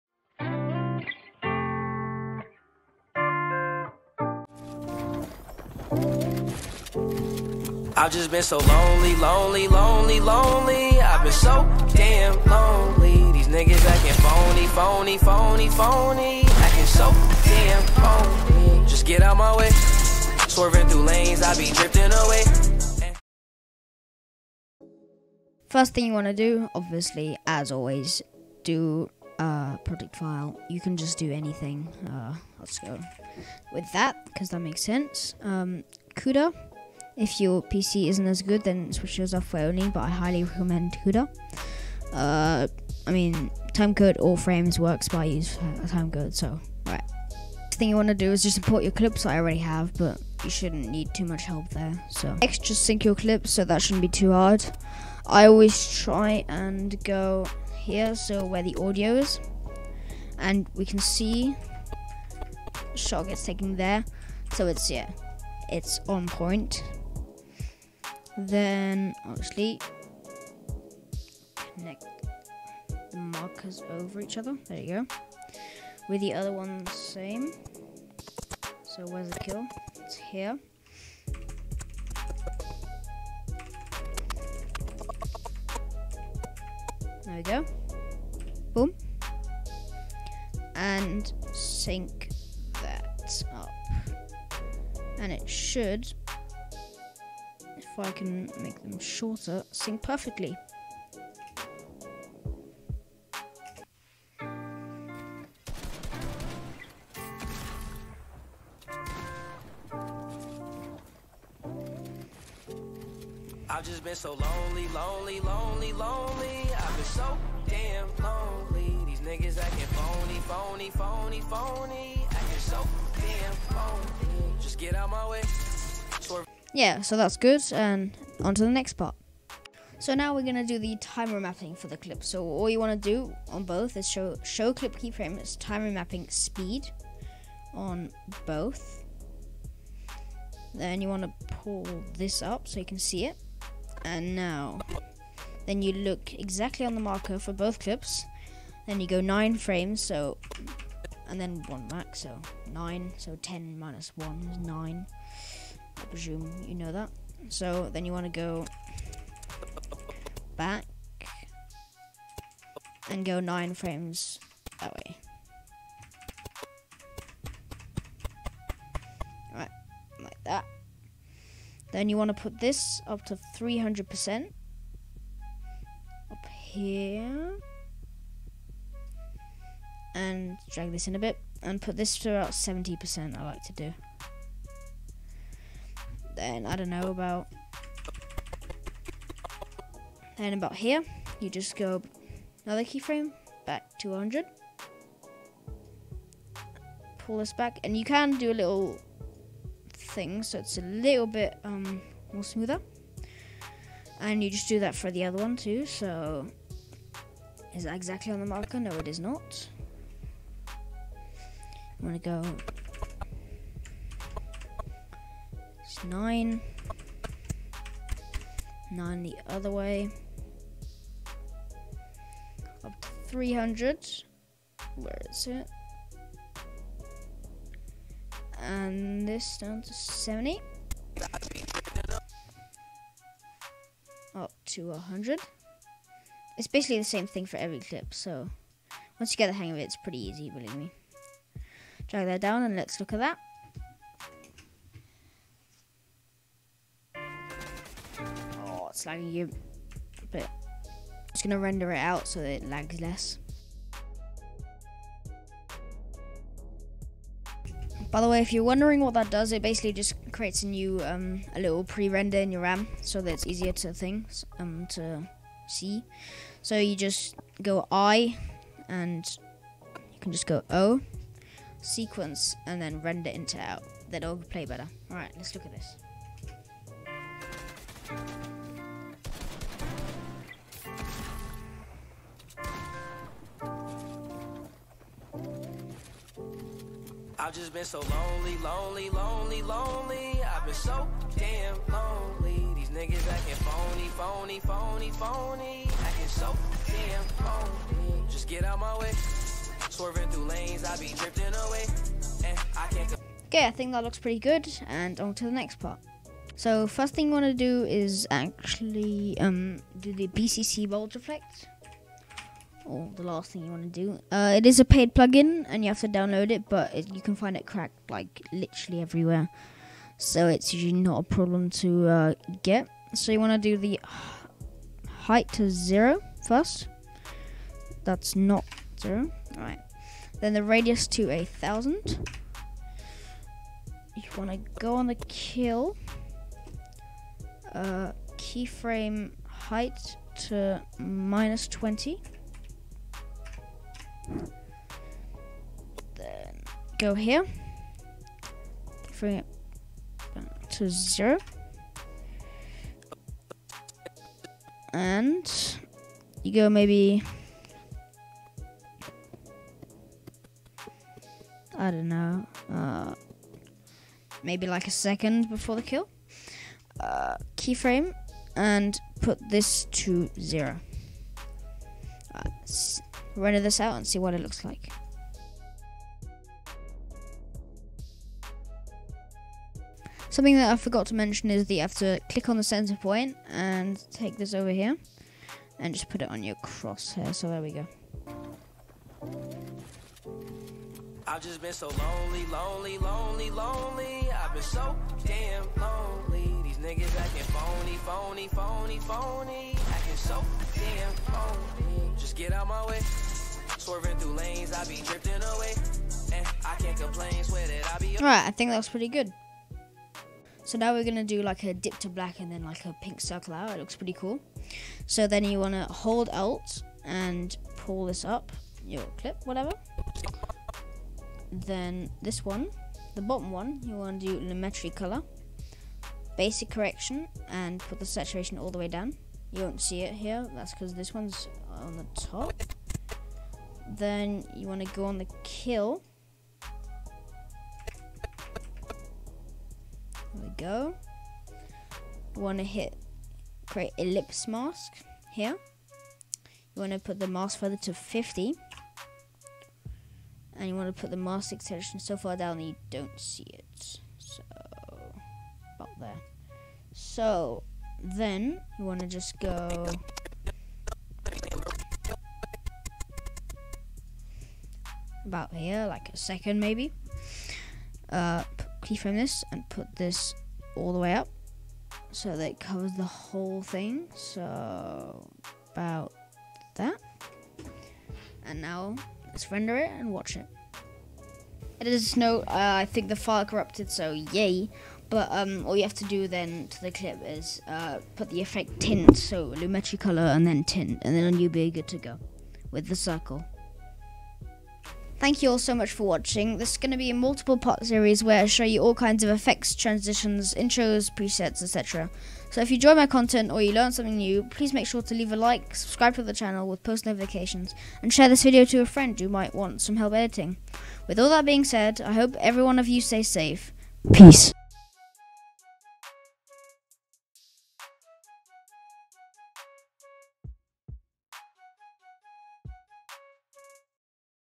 How To Edit Like LMGK, Numby and Valwop Premiere Pro (Mitr0, Mongraal, Crr, Wolfiez)
today I will show you how to do the pinch-punch and the pink circle effect for your next highlights video on premiere pro